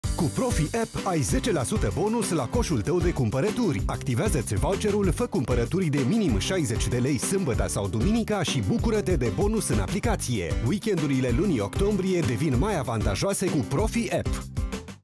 Bonus App Voucher 10% // 28 - 29 Octombrie
Nu rata ocazia de a obține 10% BONUS la coșul tău de cumpărături. Activează voucher-ul, cumpără de minim 60 de lei sâmbătă sau duminică și bucură-te de beneficii în Profi App! 🛒